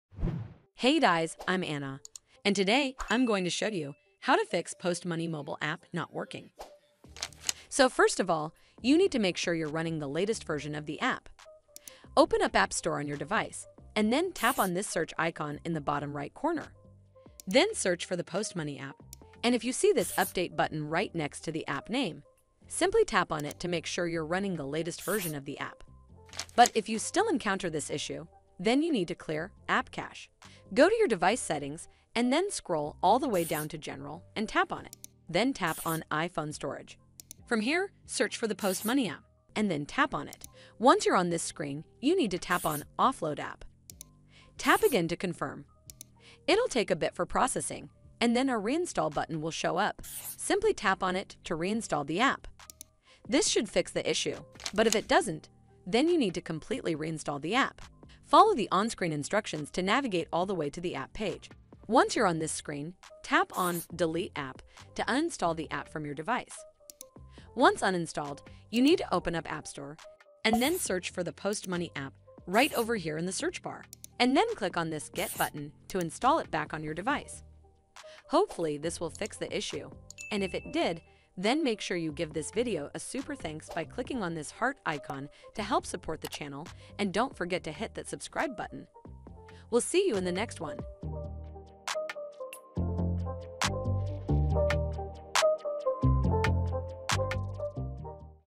An Post Money App Not Working: How to Fix An Post Money App Not Working
In this video, I'll show you How to Fix An Post Money App Not Working. This is the easiest and fastest way to Fix An Post Money App Not Working. Make sure you watch until the end of this video to find out How to Fix An Post Money App Not Working on ios. These methods work on iOS 11 as well as iOS 12, iOS 13, iOS 14, iOS 15 and iOS 16. Hope you enjoy!

Video Parts:
00:00 Intro: How to Fix An Post Money App Not Working
00:07 1.Solution: Update An Post Money App
00:27 2.Solution: Clear An Post Money App cache files
00:57 3.Solution: Delete and Reinstall An Post Money App
01:19 Outro: Ending